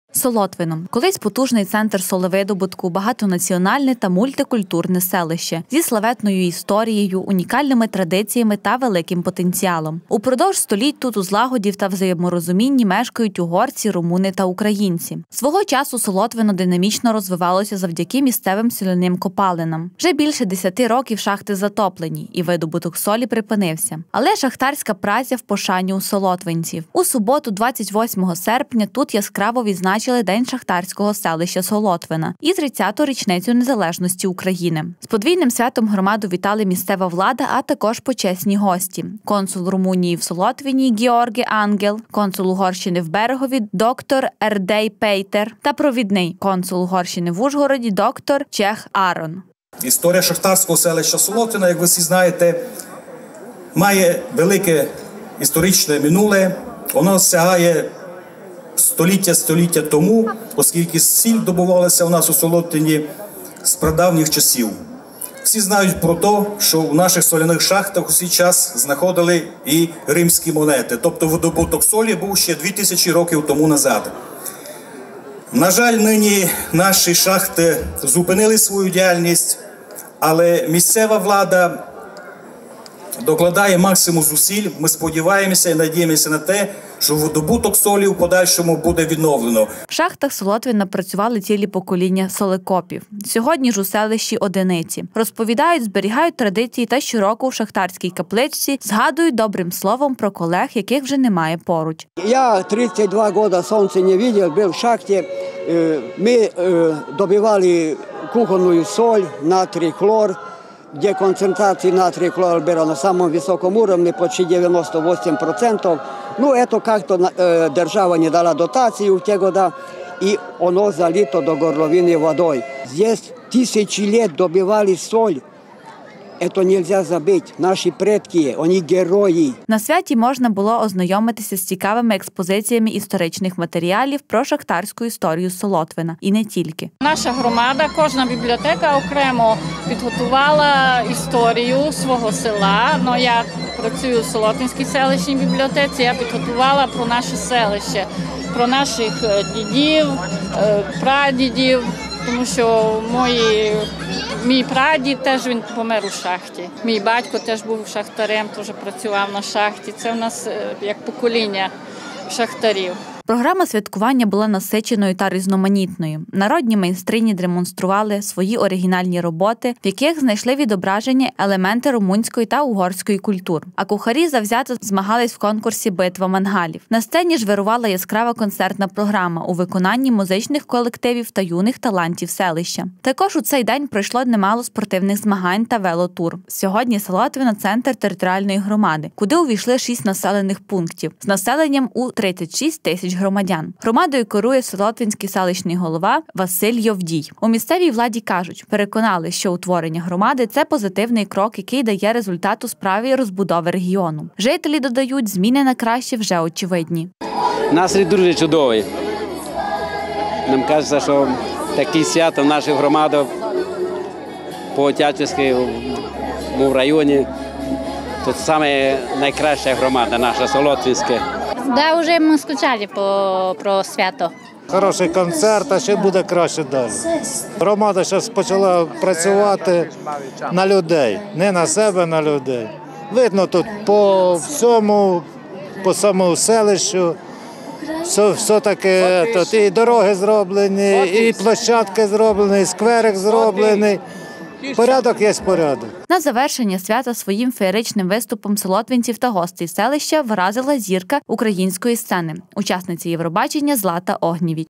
Святкувало шахтарське селище. Професійне свято
Традиційно, в останню неділю вересня в Україні відзначають День шахтаря. З цією професією тісно пов’язана історія закарпатського Солотвина, де видобували сіль ще за часів Римської імперії. В наш час видобуток білого  золота» у селищі припинено, але солотвинці щороку віддають шану предкам, які все життя трудились під землею. Тож і день селища відзначають саме цього дня. Цього року святкування збіглися у часі з 30-ю річницею Незалежності  України. Солотвино – колись потужний центр солевидобутку, багатонаціональне та мультикультурне селище, зі славетною історією, унікальними традиціями та великим потенціалом. Упродовж століть тут у злагоді та взаєморозумінні співмешкають угорці, румуни та українці. Свого часу Солотвино динамічно розвивалося завдяки місцевим соляним копальням. Вже більше десяти років шахти  затоплені, і видобуток солі припинився. Але шахтарська праця – в пошані у солотвинців.В суботу, 28 серпня, тут яскраво відзначили День шахтарсього селища Солотвина і 30-ту річницю Незалежності України. З подвійним святом громаду вітали місцева влада, а також почесні гості – Консул Румунії в Солотвині Ґеорге А́нгел (кадр 6016), Консул Угорщині в Берегові Доктор Ердей Пийтер та провідний консул Угорщини в Ужгороді Доктор Чех А́рон.У шахтах Слотвина працювали цілі покоління солекопів. Сьогодні їх у селищі – одиниці. Розповідають: зберігають традиції та щороку у шахтарській капличці згадують добрим словом про колег, яких уже немає поруч. На святі можна було ознайомитись з цікавими експозиціями історичних матеріалів про  шахтарську історію Солотвина і не тільки.Програма святкування була насиченою та різноманітною. Народні майстрині демонстрували свої оригінальні роботи, в яких знайшли відображення елементи румунської та угорської культур. А кухарі завзято змагались у конкурсі «Битва мангалів». На сцені ж вирувала яскрава концертна програма у виконанні музичних колективів та юних талантів селища. Також у цей день пройшло немало спортивних змагань та велотур. Сьогодні Солотвино – центр територіальної громади, куди увійшли 6 населених пунктів, з населенням у 36 тисяч громадян. Громадою керує Солотвинський селищний голова Василь Йовдій. У місцевій владі кажуть: переконались, що утворення громади – це позитивний крок, який дає результат у справі розбудови регіону. Жителі додають: зміни на краще вже очевидні. На завершення свята своїм феєричним виступом солотвинців та гостей селища вразила «зірка» української сцени, учасниця Євробачення Злата Огнєвіч.